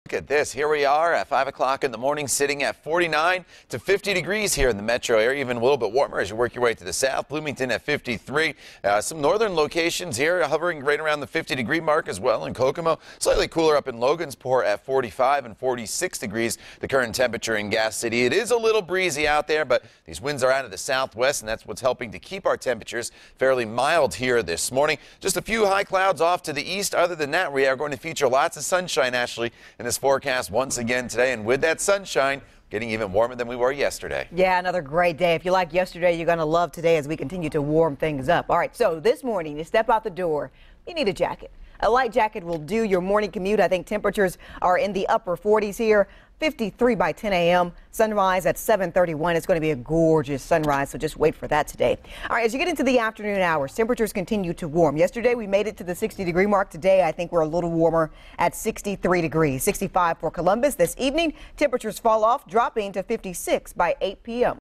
Warm, beautiful forecast on tap Tuesday
Temperatures this morning are running a little bit warmer than they have the past few mornings and the result will be warmer temperatures this afternoon as well.